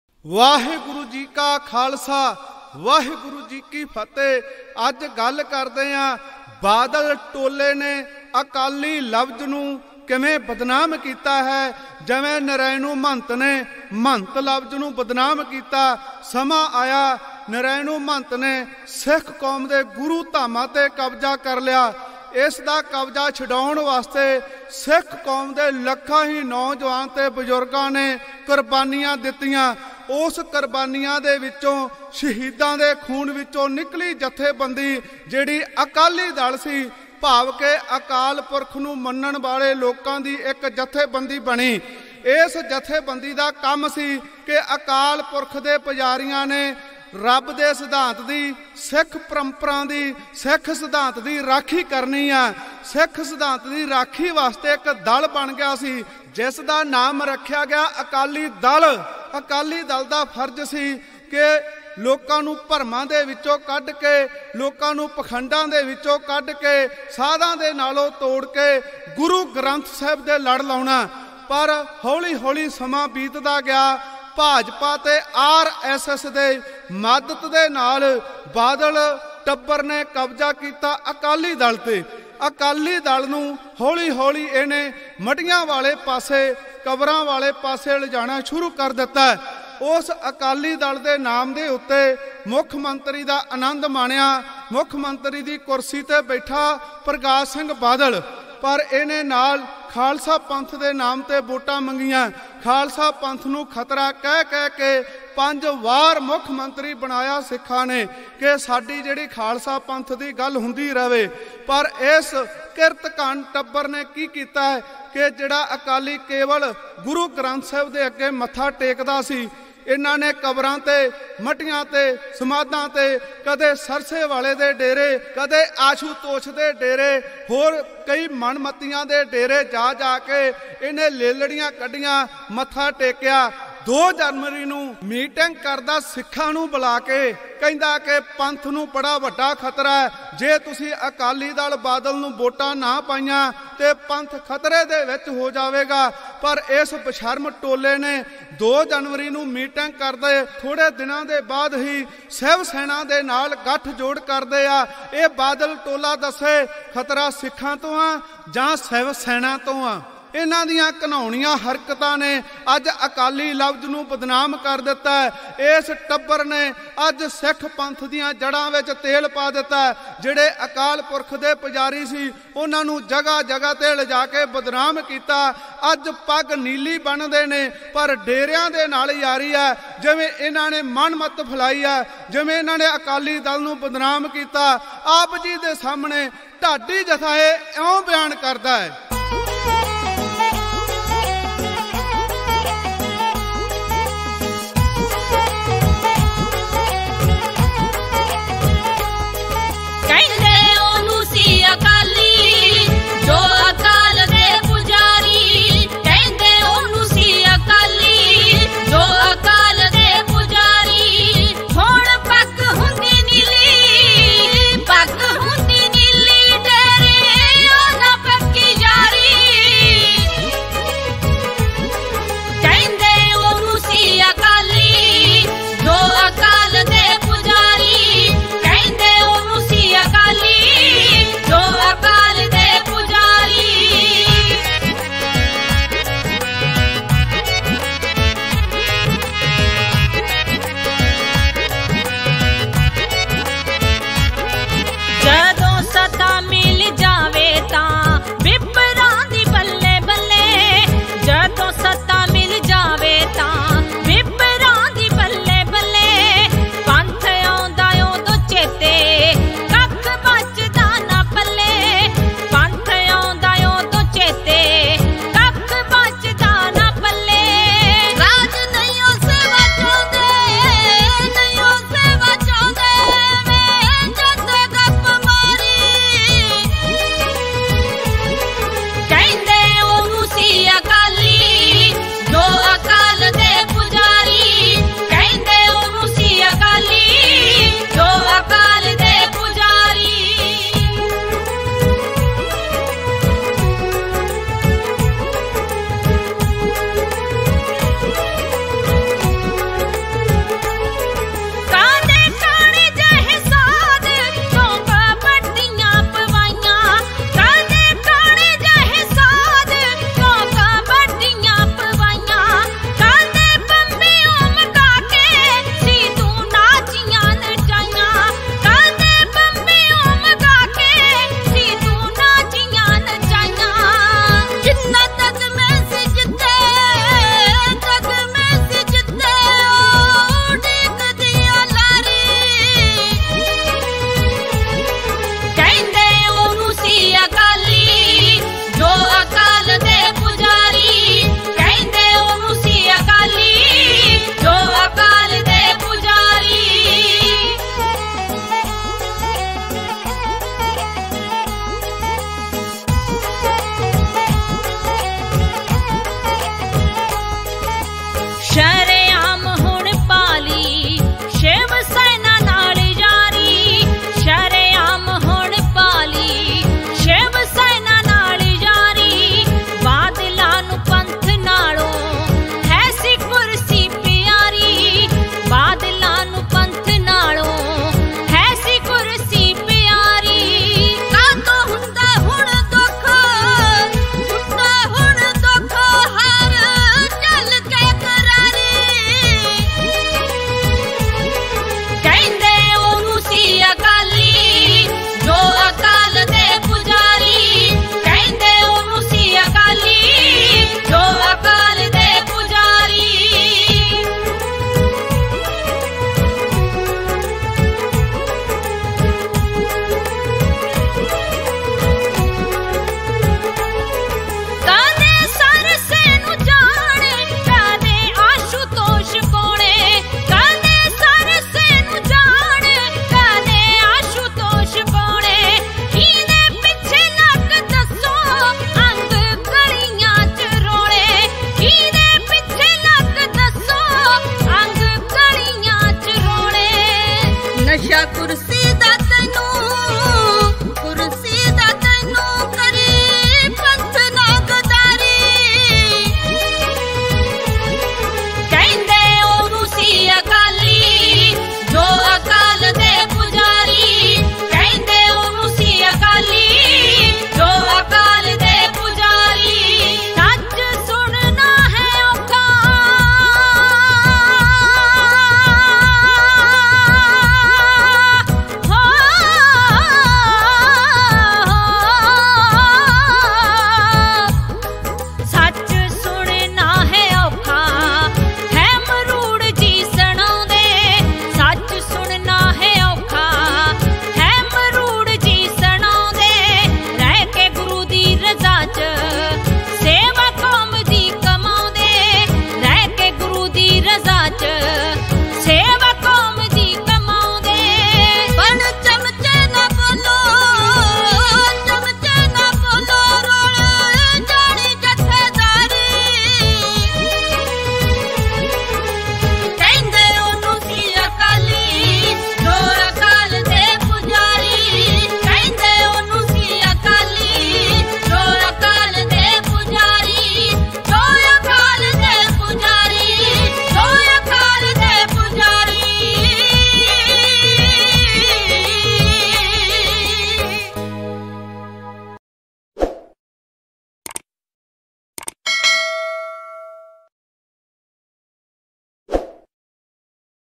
ਅਸਲੀ ਅਕਾਲੀ ਕੌਣ ? Shormni Akali Dal | Badal Family | Dhadi Jatha Gurbhag Singh Maroor | Gurmat Bibek
Dhadi Jatha
Bhai Gurbhag Singh Maroor
Dhadi Gurpreet Kaur Samadh Bhai
Dhadi Gurdeep Kaur Neetu Barnala
Sarangi Mastar Sukhwinder Singh Khalsa